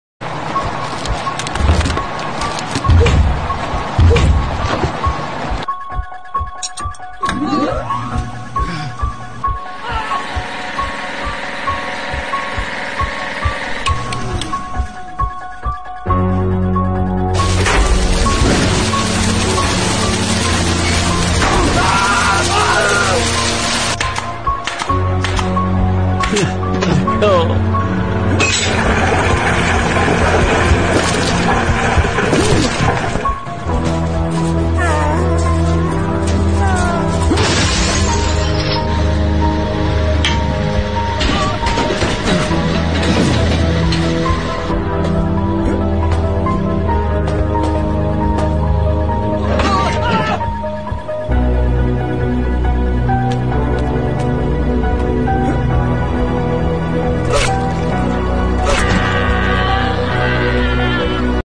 Te wypadki wydarzyły się naprawdę
Animowana wersja wypadków, które wydarzyły się naprawdę